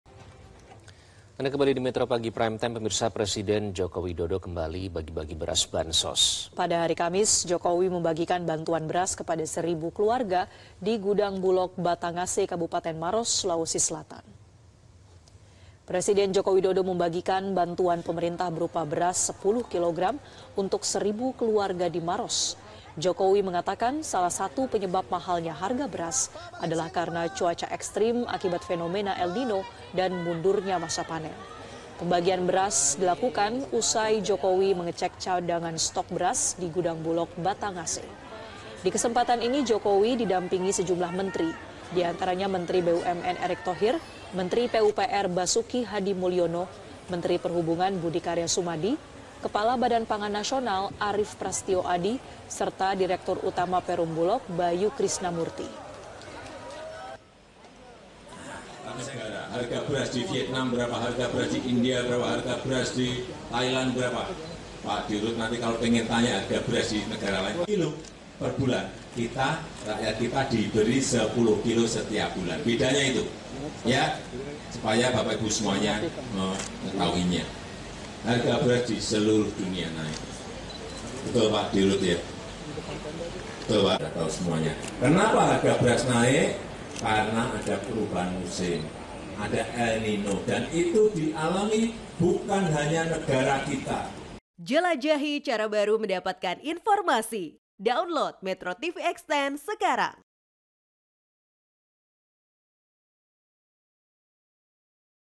Presiden Jokowi Kembali Bagi-bagi Beras Bansos
MetroTV, Presiden Joko Widodo kembali bagi-bagi beras bansos. Pada hari Kamis, 22 februari Jokowi membagi-bagikan beras kepada 1000 keluarga  di gudang Bulog Batangase Kabupaten Maros Sulawesi Selatan.